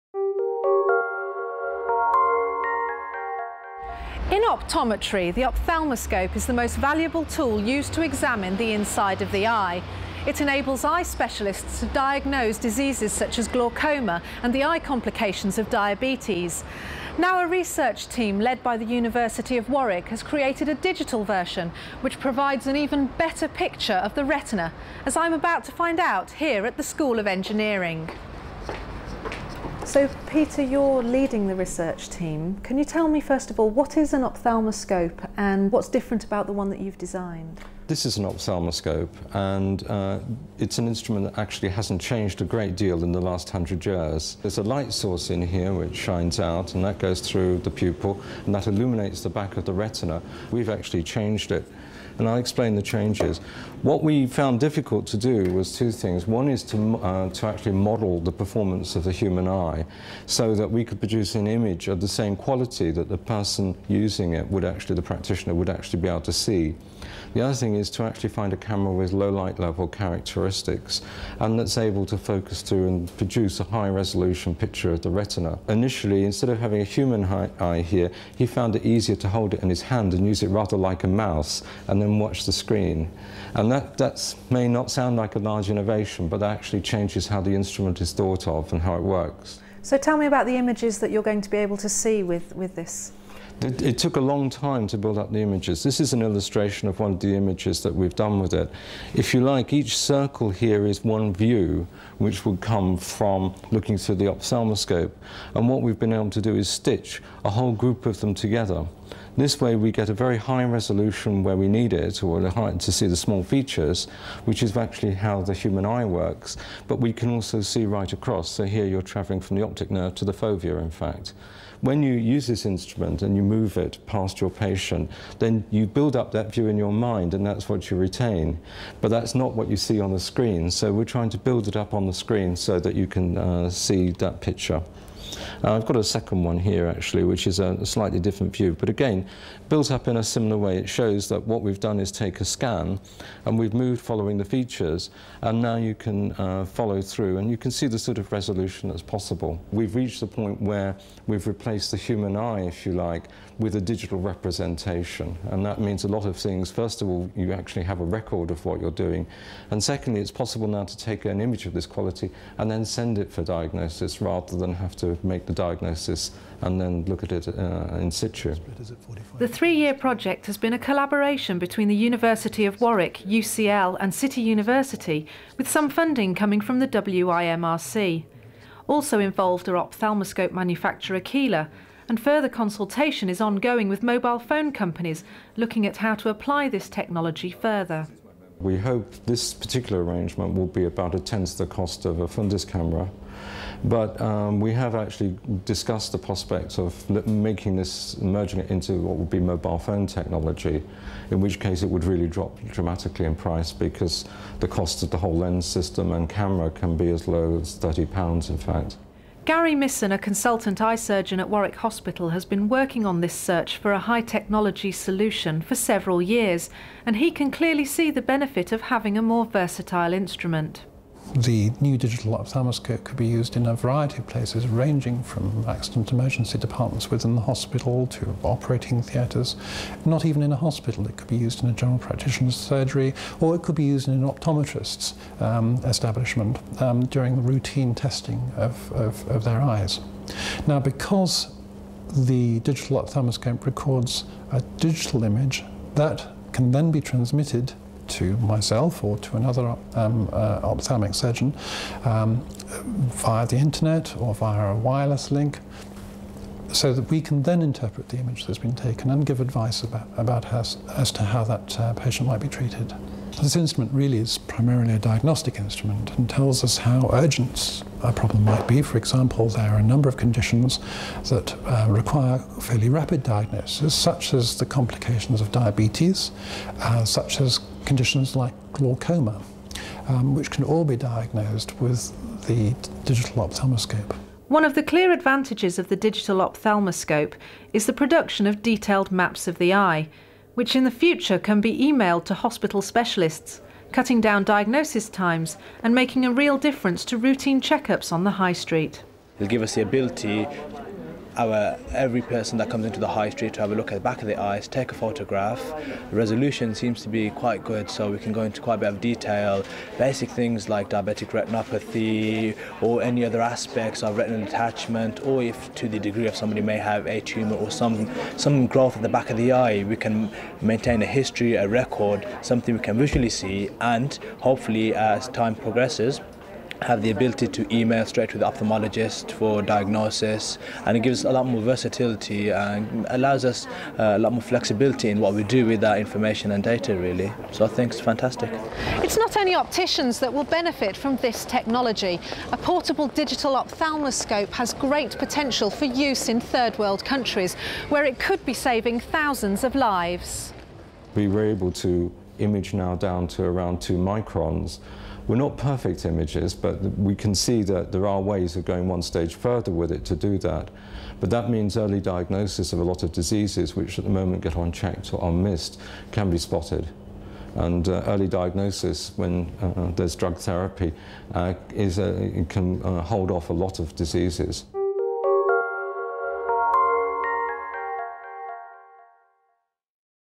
A digital opthalmascope
How the University of Warwick's Engineering Department has developed a new digital opthalmascope that could brign dramatic benefits on both the high street and in the developing world.